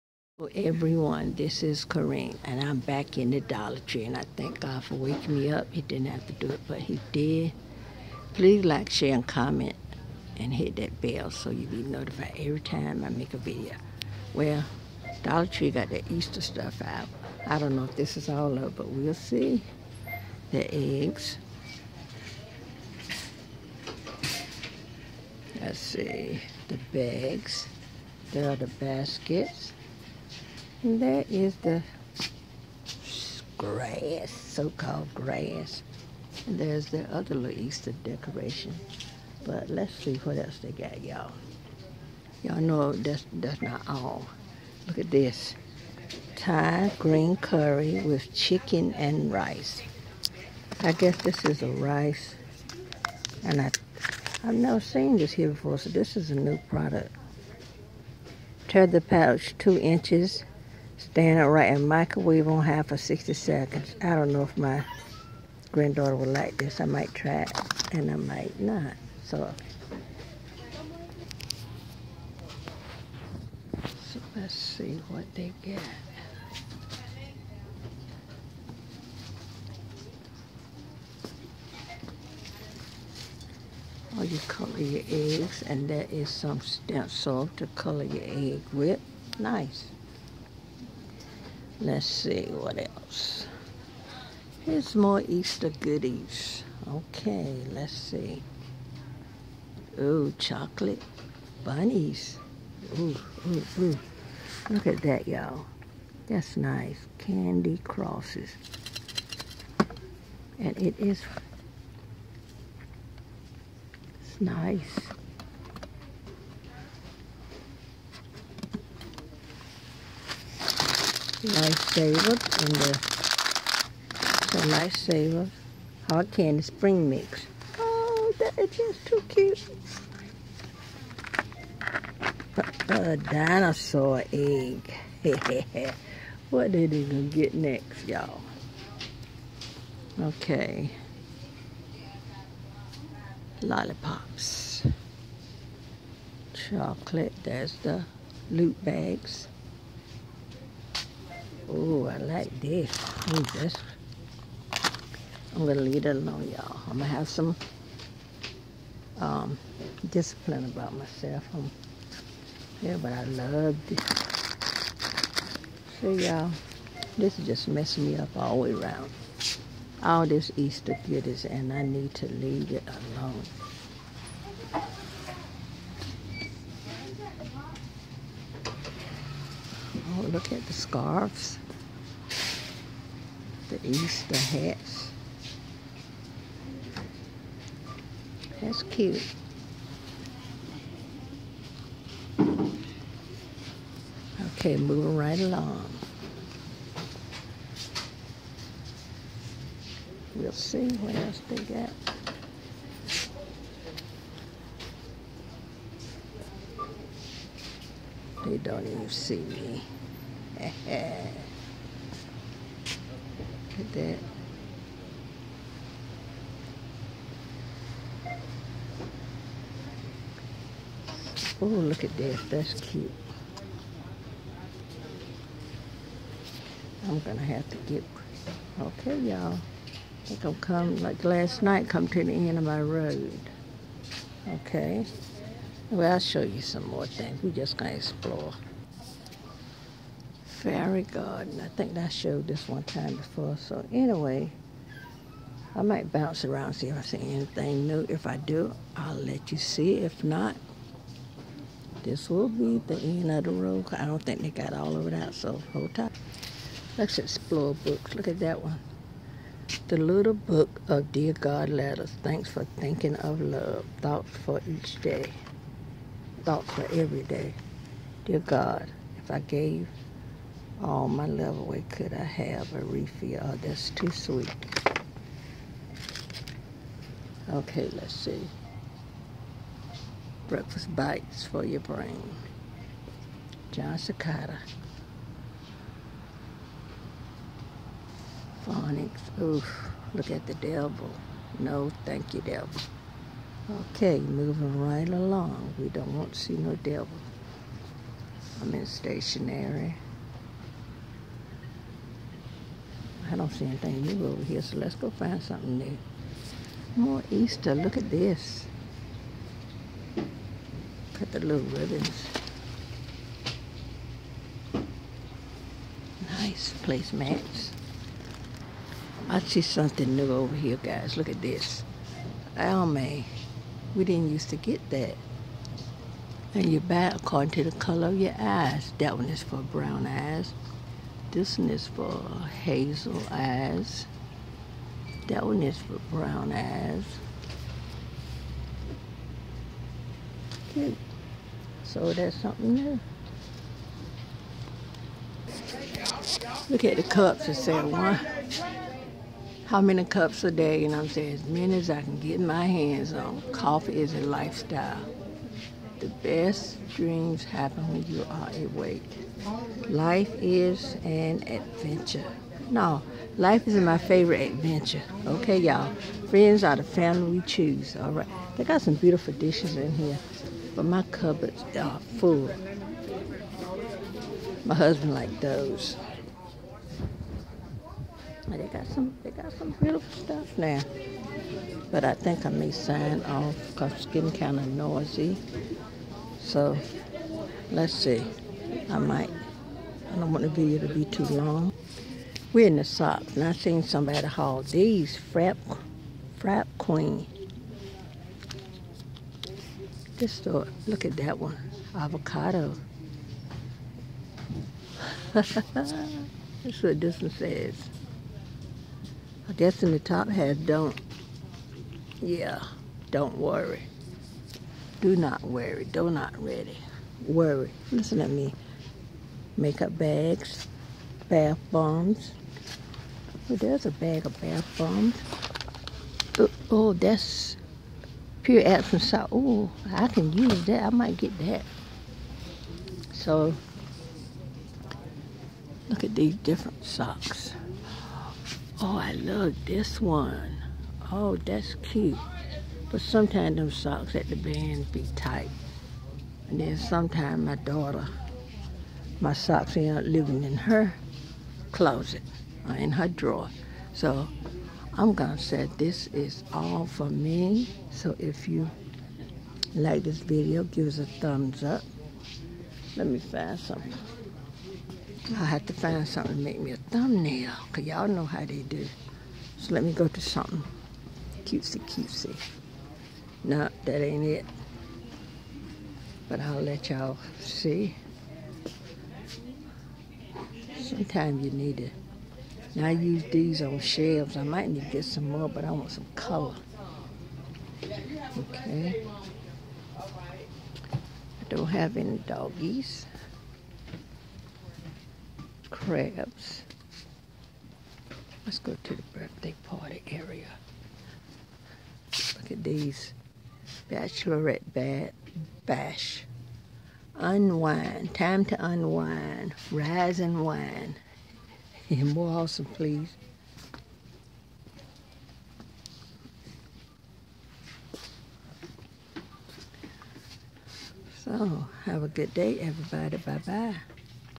Dollar Tree's Easter & my rambling LOL
I was in DT on this rainy day & since I didn't get my video up for yesterday I figured why not! !